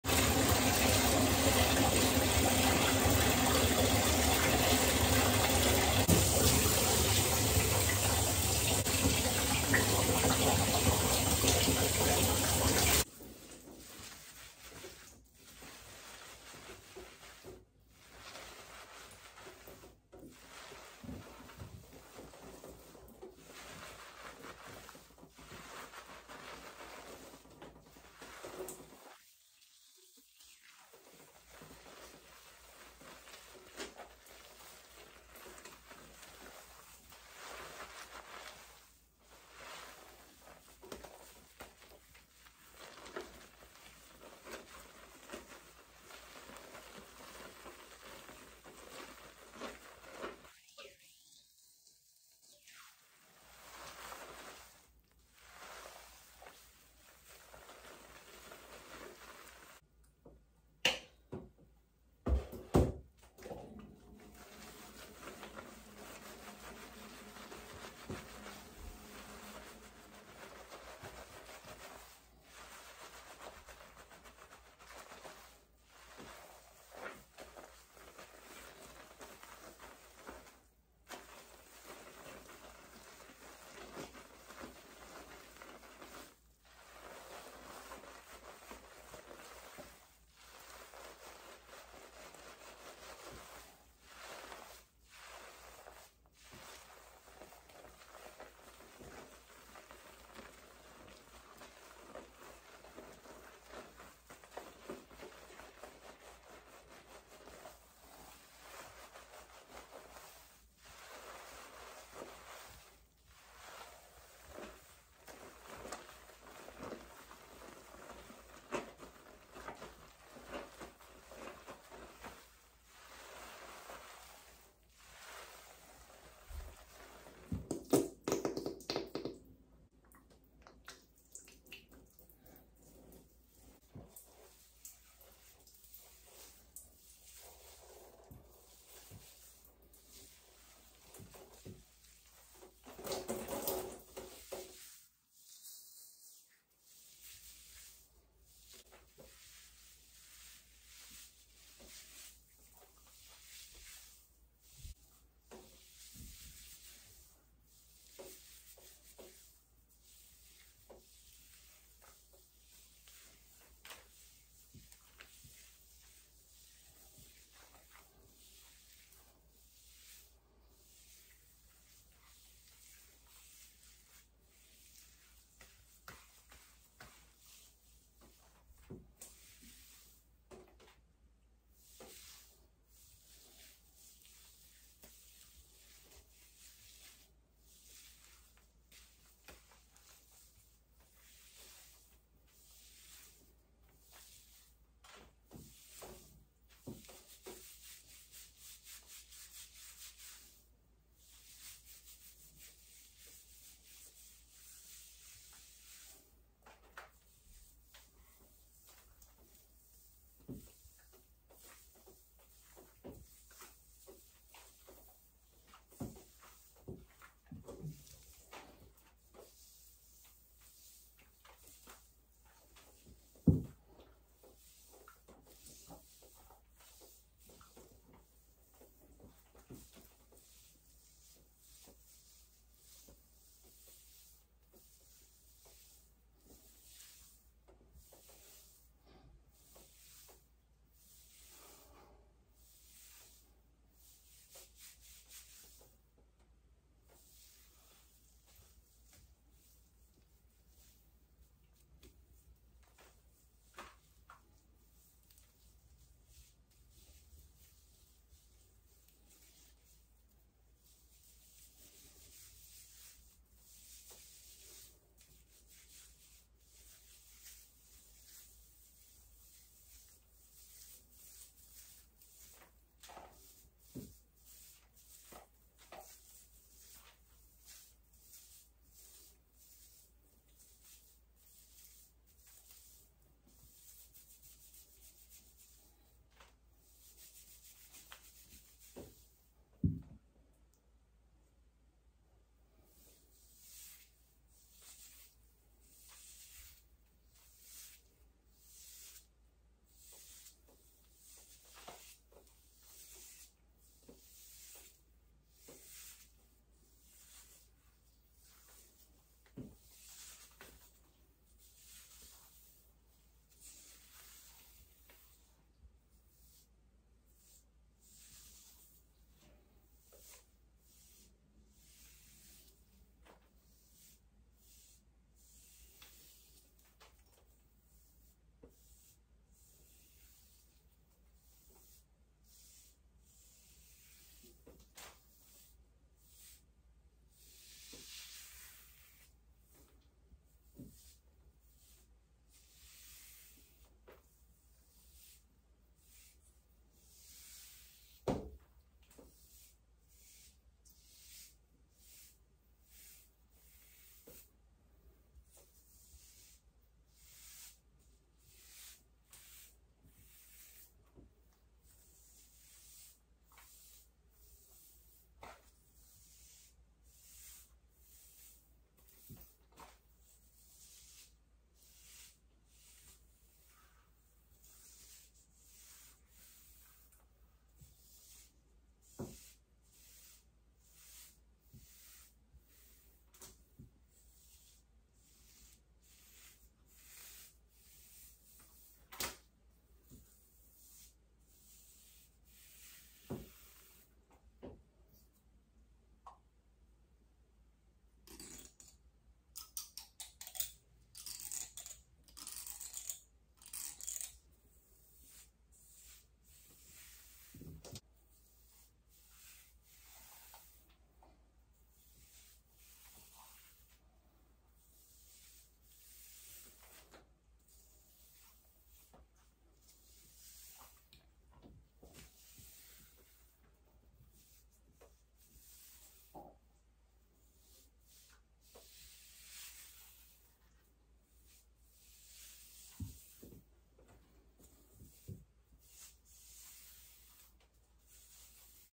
HAIR SHAMPOO & CONDITIONING ASMR
Hello People! in this video are hair dry and wet hair untill i conditioner i hope you like it..requested from two person specially sir TJ.Thank you everyone